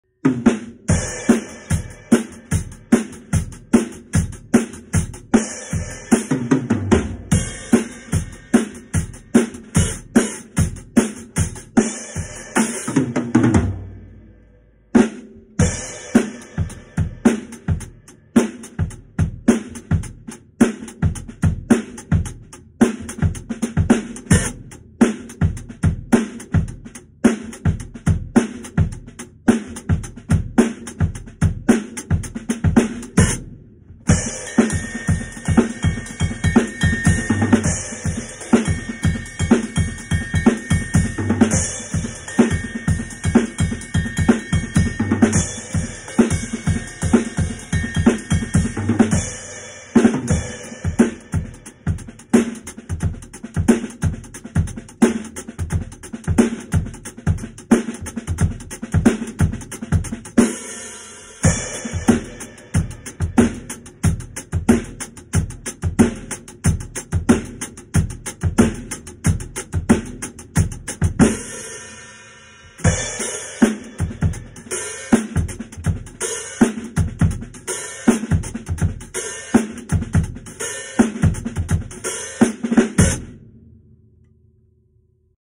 What Toto songs with Jeff Porcaro do these grooves belong to..?
Here I play 6 grooves that Jeff Porcaro plays on 6 Toto songs and which songs are they? Some are more famous and some trickier to recognize, but if you’re a huge J P fan you'll know them. I'm not playing it 100% like Jeff of course but who can? :-)

Filmed with my iPhone as usual